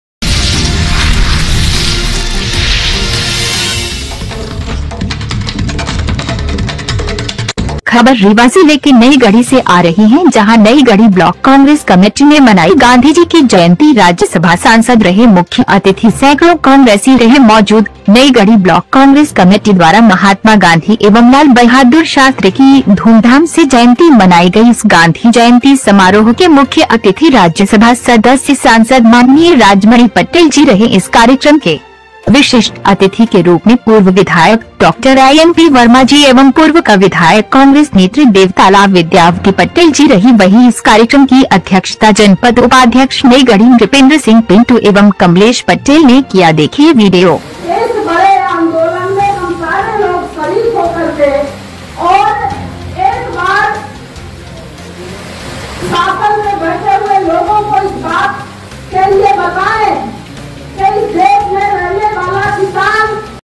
नईगढ़ी ब्लाक कांग्रेस कमेटी ने गांधी एवं लाल बहादुर शास्त्री की धूमधाम से मनाई जयंती
misirgawan news